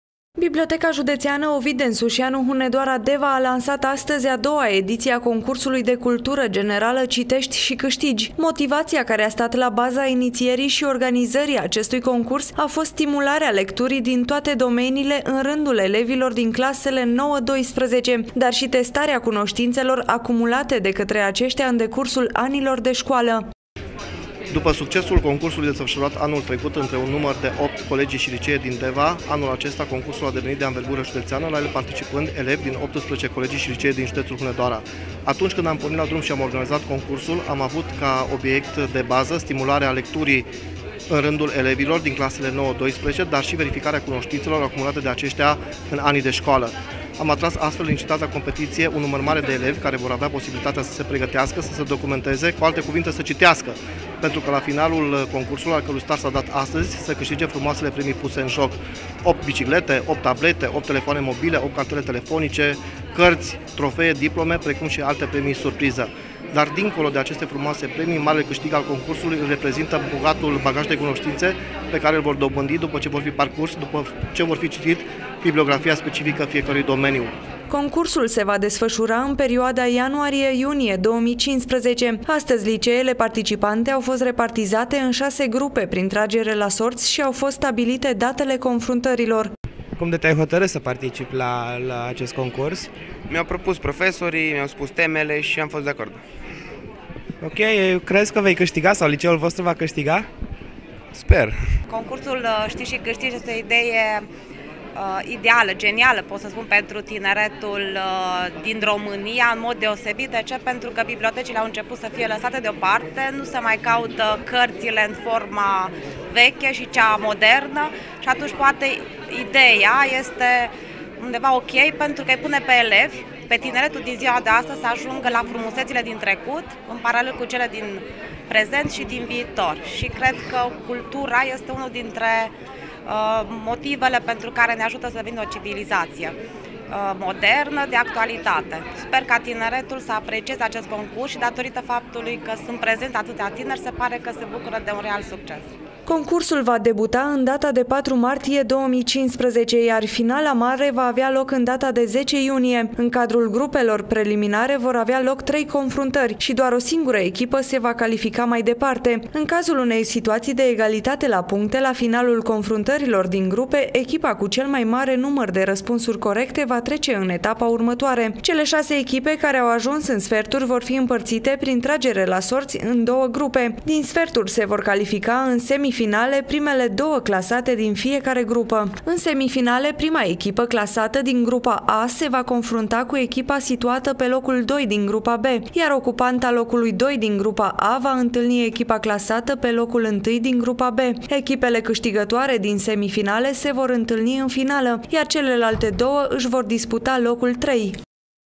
CONCURSUL „CITEȘTI ȘI CÂȘTIGI”, O NOUĂ EDIȚIE
Liceele participante au fost repartizate în șase grupe prin tragere la sorți